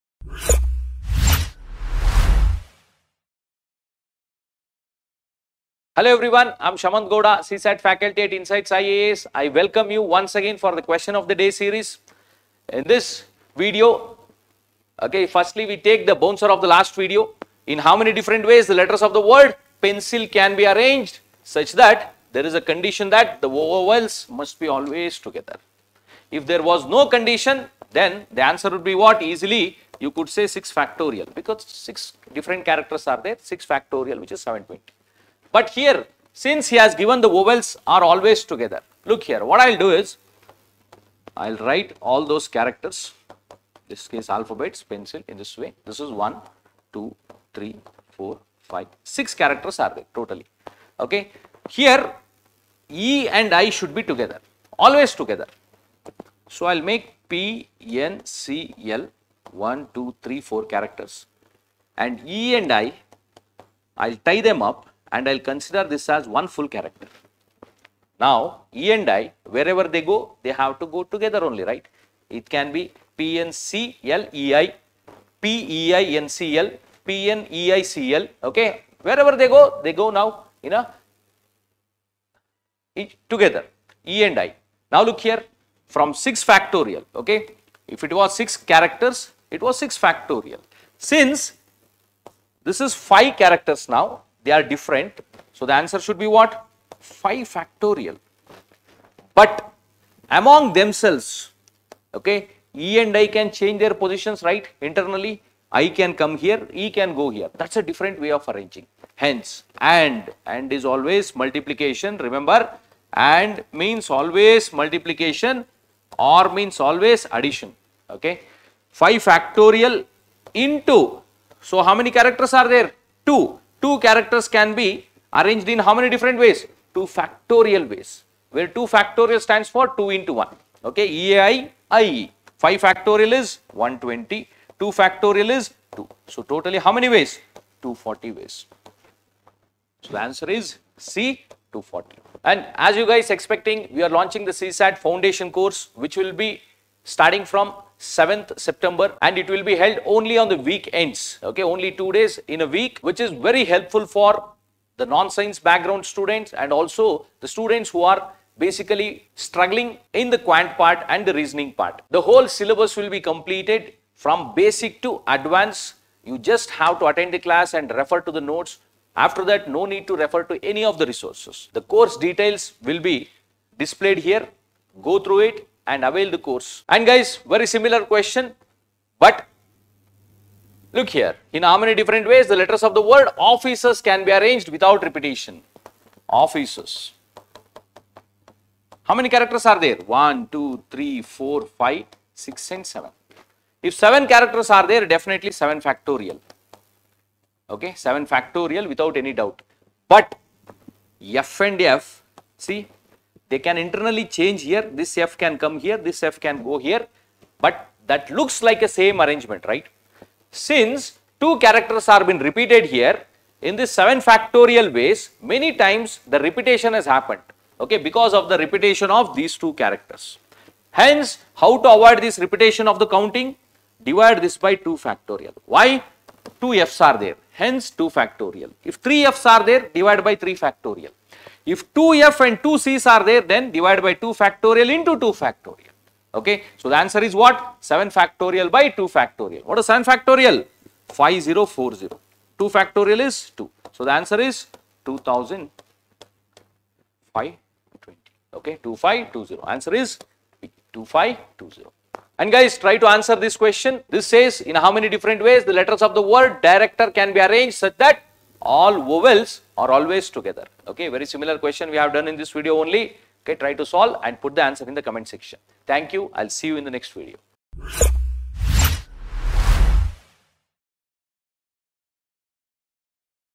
[2nd Sept 2024] CSAT Question of the Day | Shamanth Sir, Faculty at Insights IAS
CSAT - Question of the Day series. In this series, Shamanth sir will be solving a question along with explanation of the topic.
We hope this series will help you learn a topic every day and learn to solve the questions at ease .